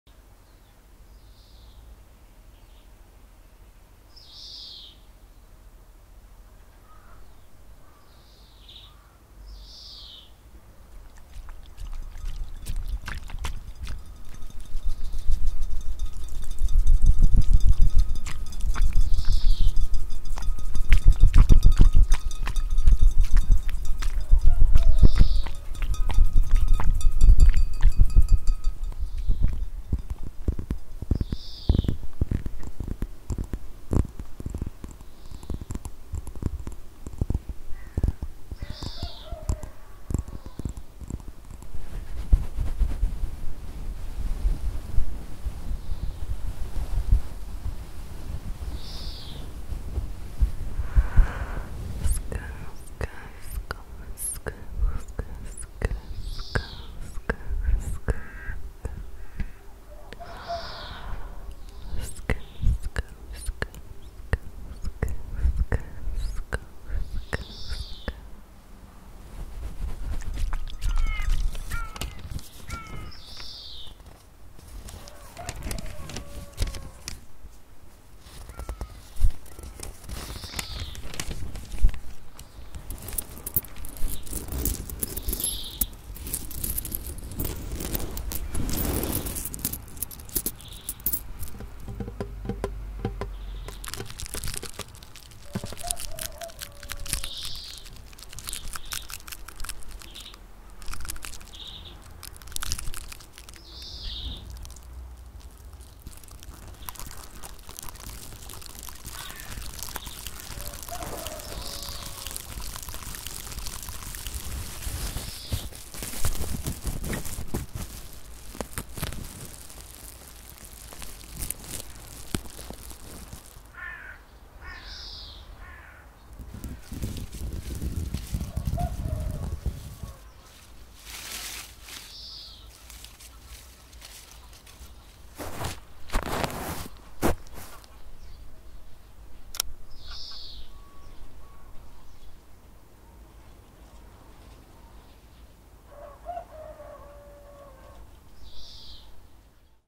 ASMR Animation - Under the Surface 😌 Tingly Brain Melting Sounds (Brain Massage for Sleep)
This ASMR brain massage animation video is designed to help you relax and unwind. Through gentle visual and audio triggers, you'll experience a soothing sensation that can help to relieve stress and promote a sense of calm. Watch now to indulge in a calming and tranquil experience.

Time Stamps:
00:00 Intro
00:11 Bottle - Water Sounds
00:29 Dinosaur - Tapping
00:39 Fox - Fur
00:51 Badger - Sk Sk Sk
01:11 Mole - Page Turning
01:23 Chest of Gold Coins
01:33 Firebugs - Crinkles
01:45 Ants - More Crinkles
01:55 Mouse - MORE Crinkles
02:05 Grain